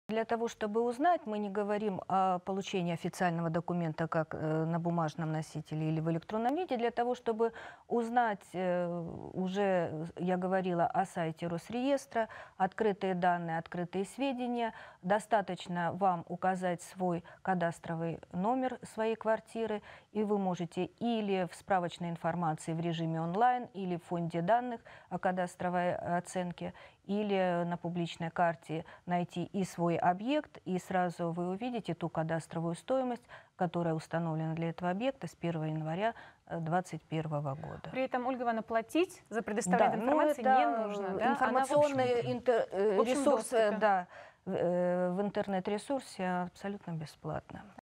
Где узнать кадастровую стоимость квартиры
В 2020 году в Белгородской области провели кадастровую оценку объектов капитального строительства. Это происходит раз в пять лет и влияет на налогооблагаемую базу, исходя из стоимости вашего имущества. Узнать кадастровую стоимость недвижимости можно в режиме онлайн. Информация предоставляется бесплатно.

В прямом эфире телеканала «Мир Белогорья» разъяснения по этому вопросу дала первый заместитель начальника департамента имущественных и земельных отношений Ольга Костюченко.